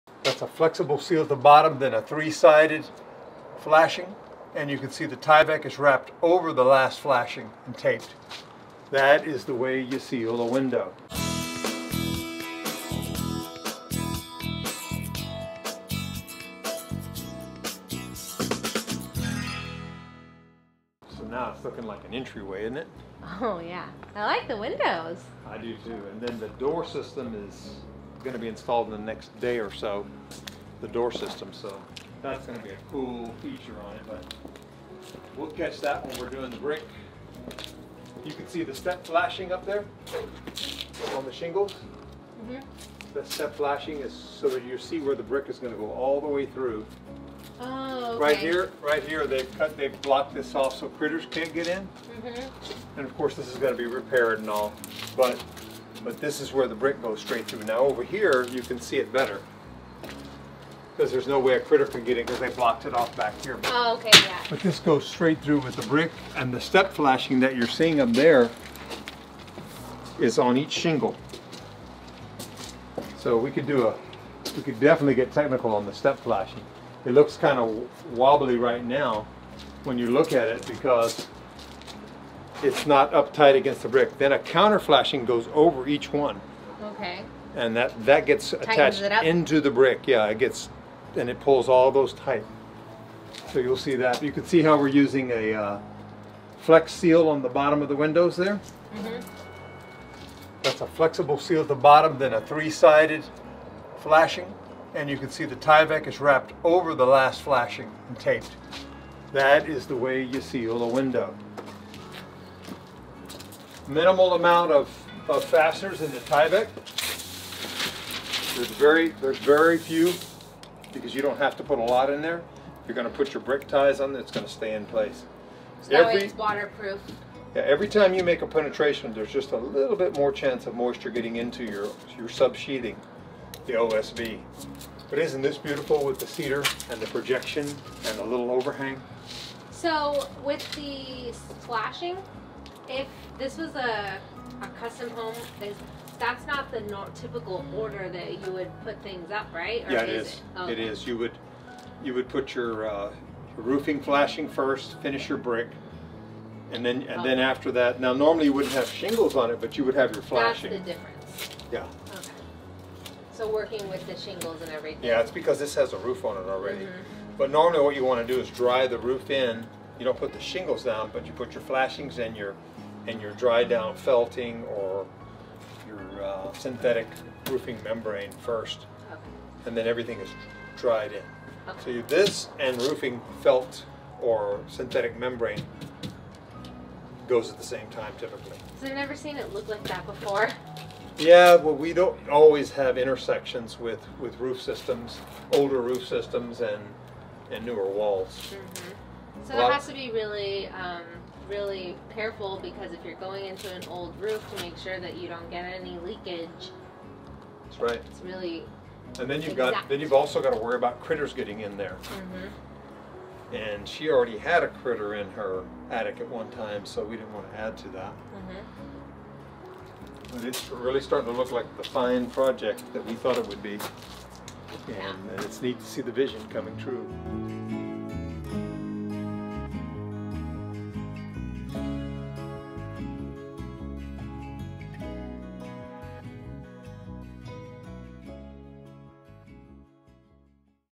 Whispering Oaks Front Entry Addition Ep. 131
The new design of this beautiful front entry is starting to take shape! Continue watching as Keith explains the importance of step flashing and how it helps to prevent any critters or moisture from getting into the OSB.